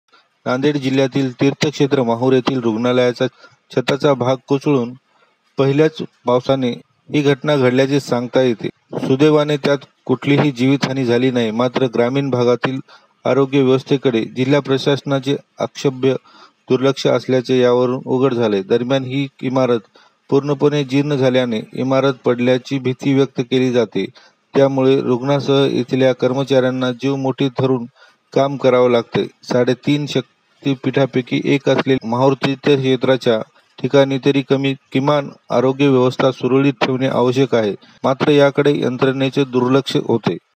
नांदेड जिल्ह्यातील तीर्थक्षेत्र माहुर येथील रुग्णालयाच्या छताचा भाग कोसळलाय
नांदेड जिल्ह्यातील तीर्थक्षेत्र माहुर येथील रुग्णालयाच्या छताचा भाग कोसळलाय. पहिल्याच पावसाने ही घटना घड़ल्याचे सांगण्यात येतय. सुद्वैवाने यात कुठलीही जीवितहानी झाली नाही. मात्र ग्रामीण भागातील आरोग्य व्यवस्थेकड़े जिल्हा प्रशासनाचे अक्षम्य दुर्लक्ष असल्याचे यावरुन उघड़ झालय. दरम्यान ही इमारत पुर्ण पणे जीर्ण झाल्याने इमारत पडण्याची भीती व्यक्त केल्या जातेय. त्यामुळे रुग्णासह इथल्या कर्मचाऱ्यांना जीव मुठित धरून काम कराव लागतय. साडे तीन शक्ती पीठापैकी एक असलेल्या माहुर तीर्थक्षेत्राच्या ठिकाणी तरी किमान आरोग्य व्यवस्था सुरळीत ठेवन आवश्यक आहे, मात्र त्याकडे यंत्रनेच दुर्लक्ष होतय